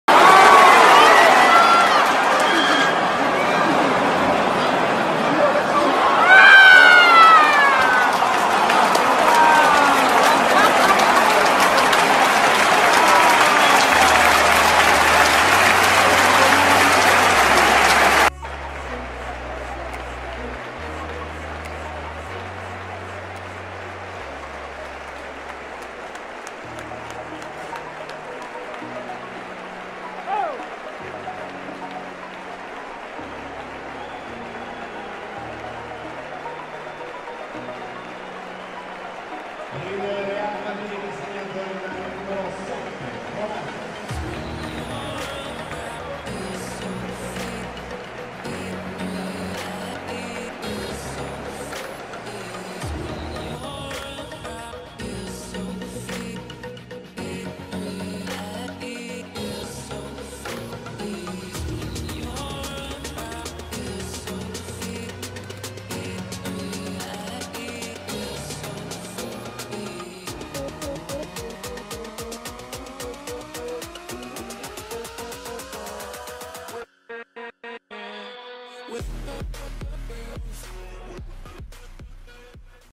ردة فعل زيدان و الجماهير بعد هدف رونالدو الاسطوري
رونالدو ضد يوفينتوس
ريال مدريد و يوفينتوس
ملخص مباراة ريال مدريد و يوفينتوس
تصفيق الجماهير رونالدو